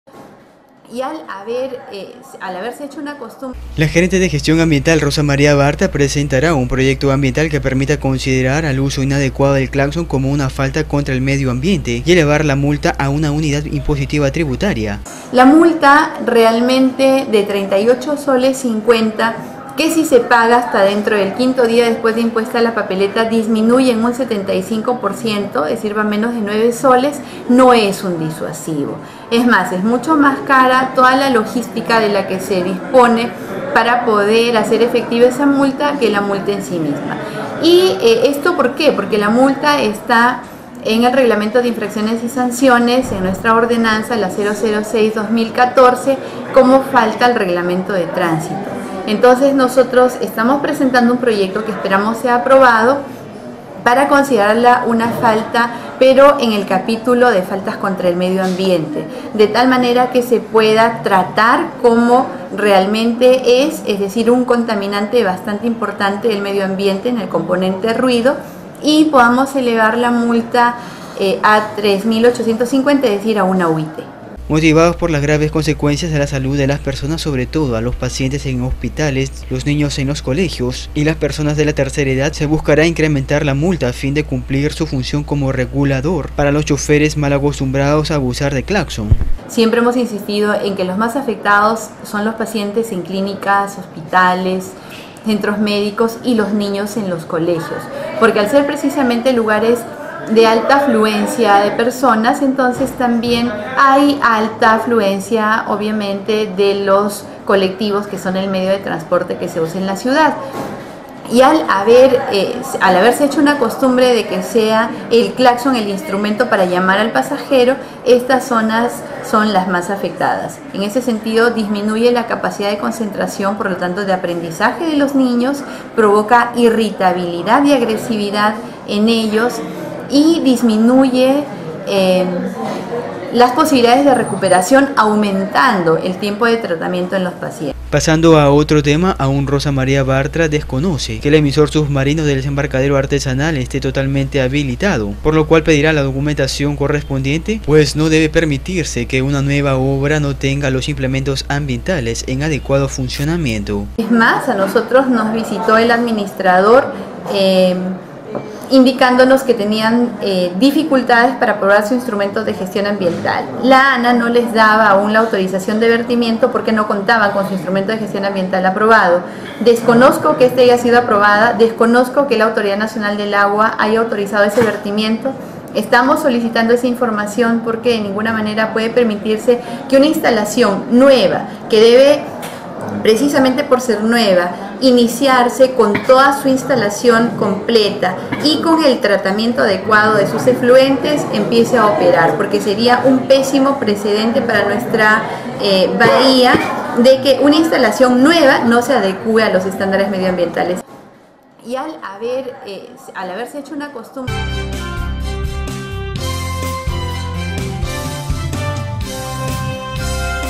INTENTAN AUMENTAR MULTA
La Gerente de Gestión Ambiental, Rosa María Bartra, presentará un proyecto ambiental que permita considerar al uso inadecuado del claxon como una falta contra el medio ambiente y elevar la multa de 15% a 1 UIT, es decir, a 3850 soles.

Se buscará incrementar la multa a fin de cumplir su función como regulador para los chóferes malacostumbrados a abusar del claxón.